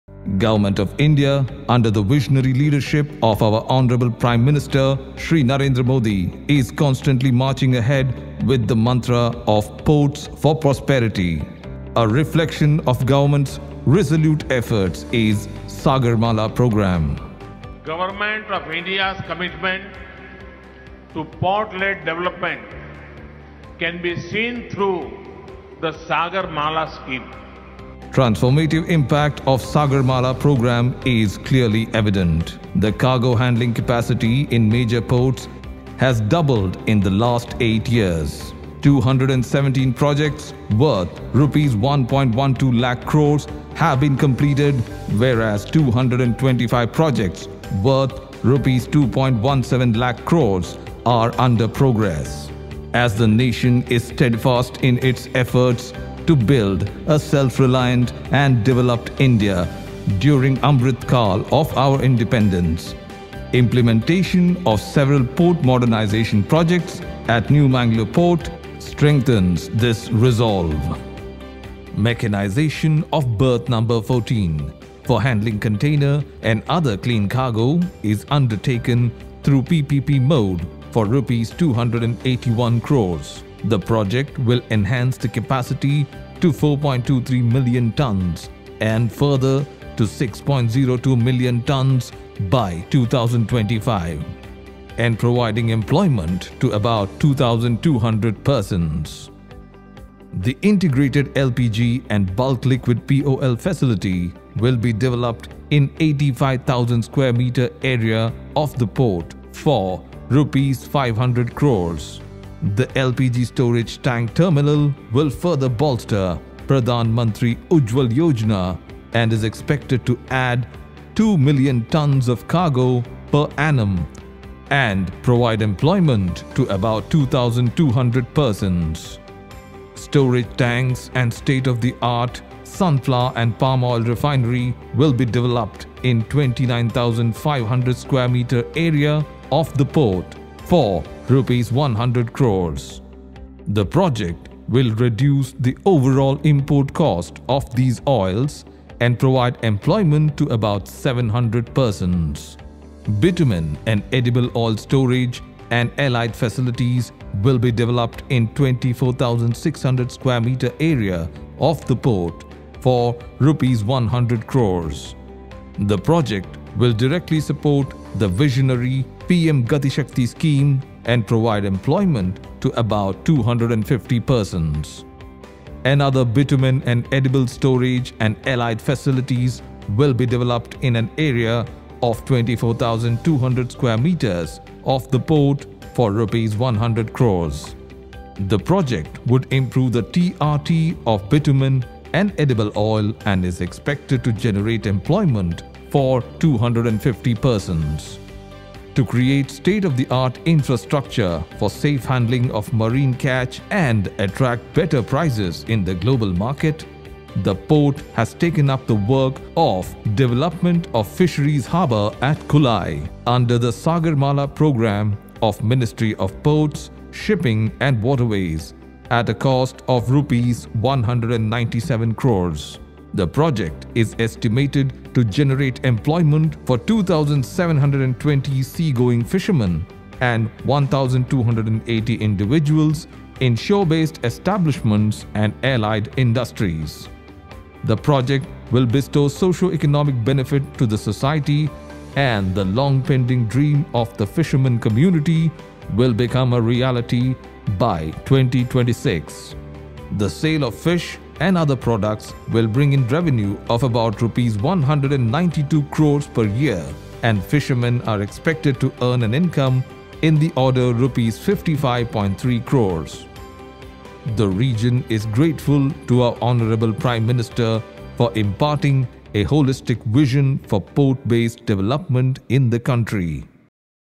Projects of New Mangalore Port Authority launched by PM Modi – All details here
Prime Minister Narendra Modi inaugurated the project worth over Rs 280 crore for mechanisation of Berth No. 14 for handling containers and other cargo, undertaken by the New Mangalore Port Authority. The mechanized terminal will increase efficiency and reduce turnaround time, pre-berthing delay and dwell time in the Port by around 35%, thus giving a boost to the business environment. Phase I of the project has been successfully completed, thereby adding over 4.2 MTPA to the handling capacity, which would further increase to over 6 MTPA by 2025.

Prime Minister Modi laid the foundation stone of five projects worth around Rs 1000 crore, undertaken by the Port. The integrated LPG and Bulk Liquid POL Facility, equipped with state of the art cryogenic LPG storage tank terminal, will be capable of unloading full load VLGC (very large gas carriers) of 45,000 tonnes in a highly efficient manner. The facility will bolster Pradhan Mantri Ujjwala Yojana in the region while reinforcing the port’s status as one of the top LPG importing ports in the country. Prime Minister laid the foundation stone of projects for construction of storage tanks and edible oil refinery, construction of bitumen storage and allied facilities and construction of bitumen & edible oil storage and allied facilities. These projects will improve the turnaround time of bitumen and edible oil vessels and reduce the overall freight cost for trade. The PM also laid the foundation stone for development of Fishing Harbour at Kulai, which will facilitate safe handling of fish catch and enable better prices in the global market. This work will be undertaken under the umbrella of the Sagarmala Programme, and will result in significant socio-economic benefits for the fishermen community.